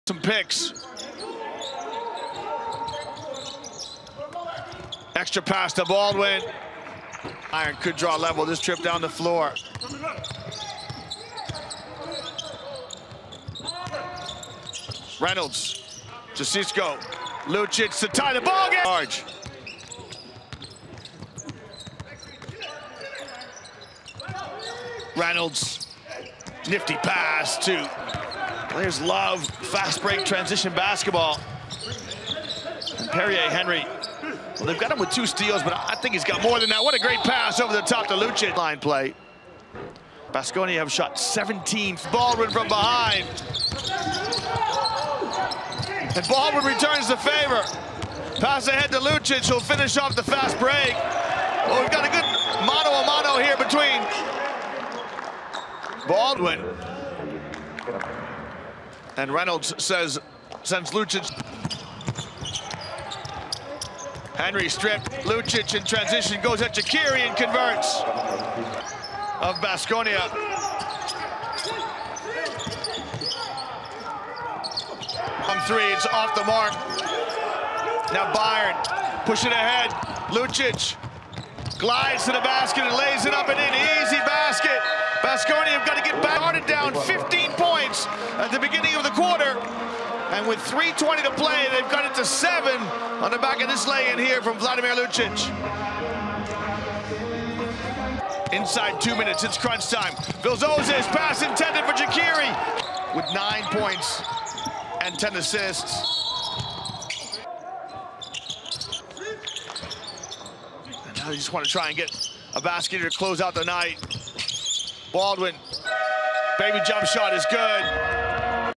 VLADIMIR LUCIC 17 points 6 rebs | Baskonia - Bayern Munich | Full Highlights | 12.03.2021
Vladimir Lucic 17 points, 6 rebounds vs Baskonia
17 poena protiv Baskonije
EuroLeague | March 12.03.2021
BASKONIA VITORIA-GASTEIZ 78 - 71 BAYERN MUNICH